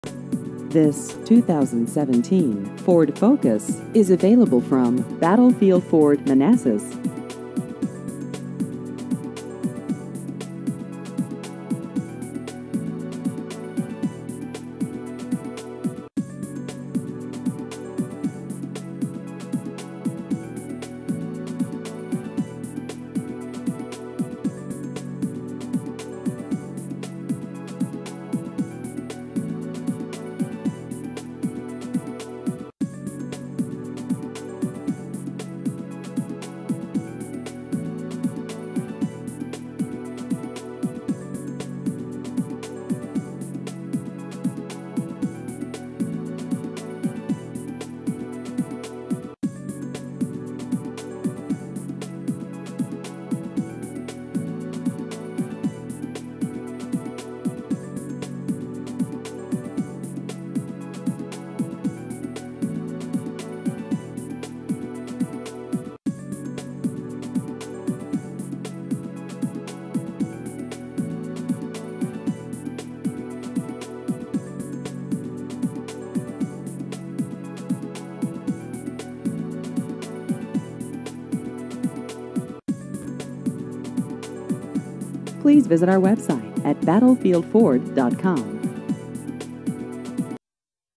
2017 Ford Focus New Cars Manassas VA
This 2017 Ford Focus is available from Battlefield Ford Manassas.